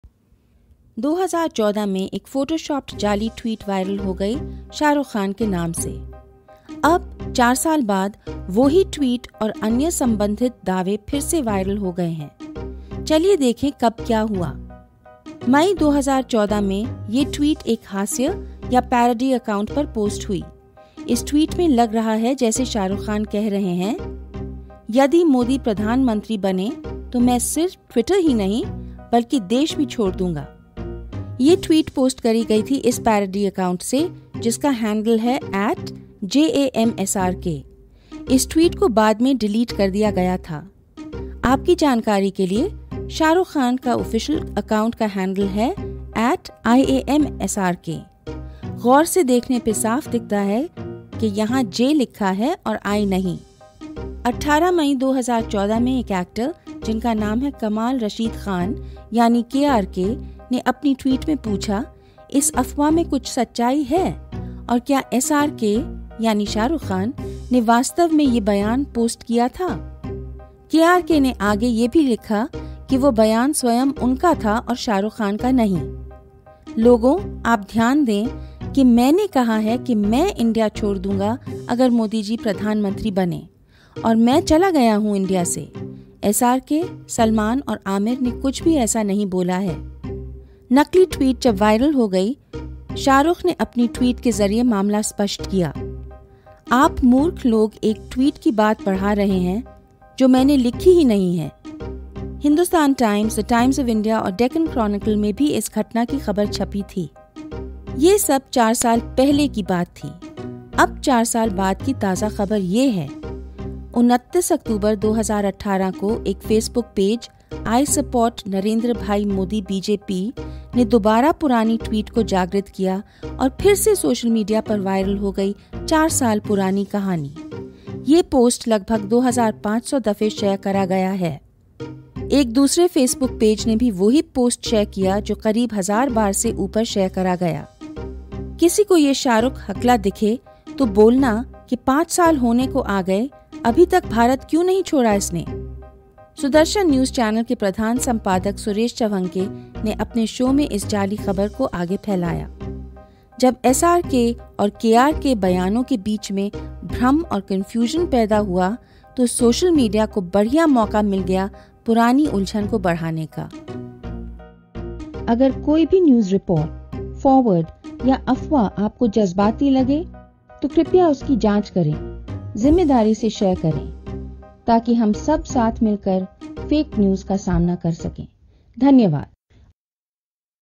क्या शाहरुख खान ने कहा कि मोदी प्रधानमंत्री बने तो वह भारत छोड़ देंगे? नकली ट्वीट वायरल
क्या शाहरुख खान ने कहा कि मोदी प्रधानमंत्री बने तो वह भारत छोड़ देंगे? नकली ट्वीट फिर से किया गया वायरल